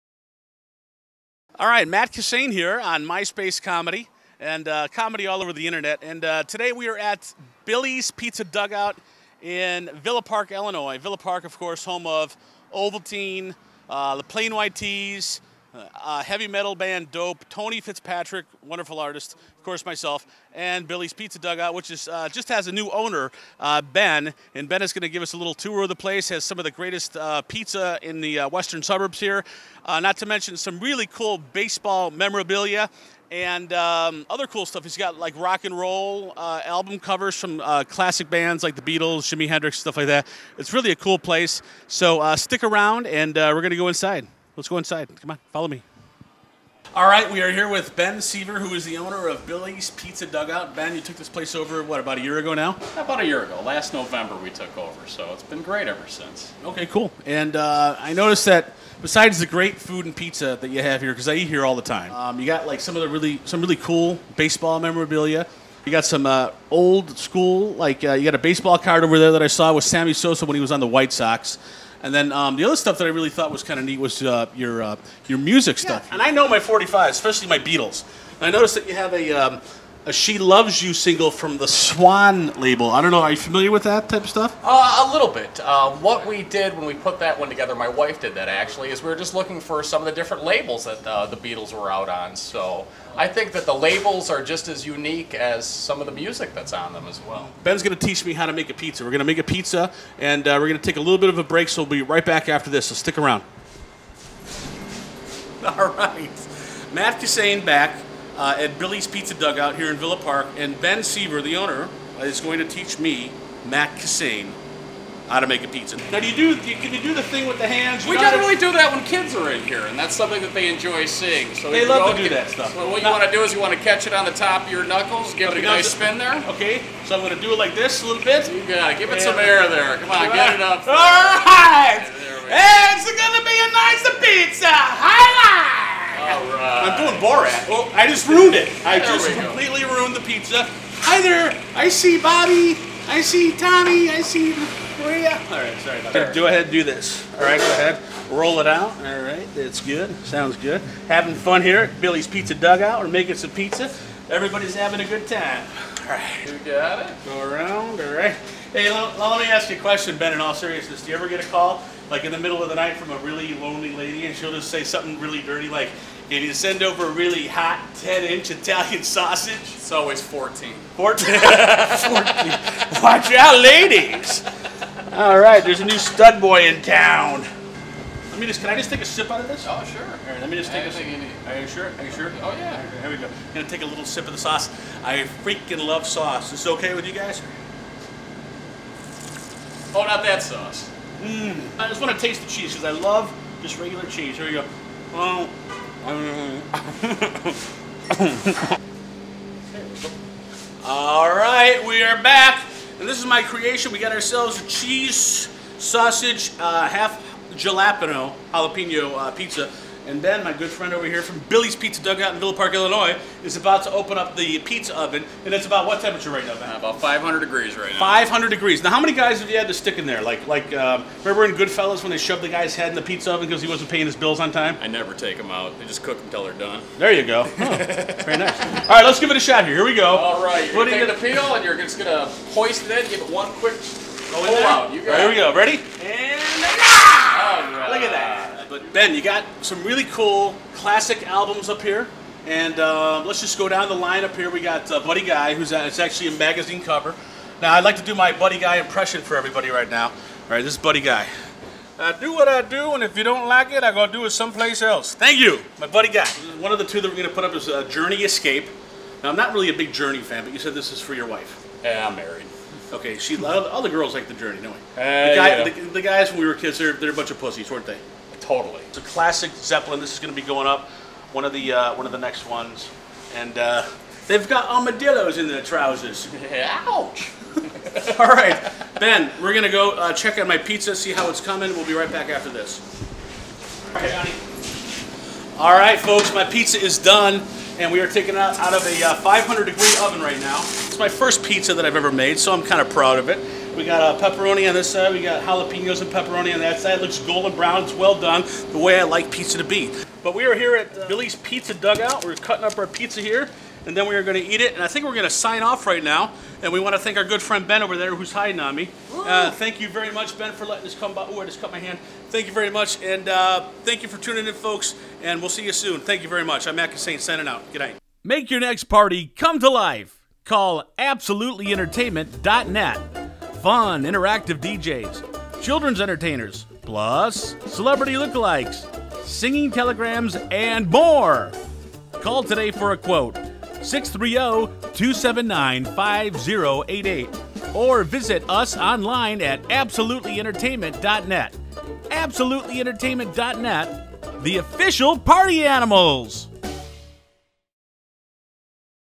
Matt Kissane visits Billy's Pizza Dugout!
Another installment of Matt's web series, this time making pizza at Billy's Pizza Dugout in Villa Park , IL!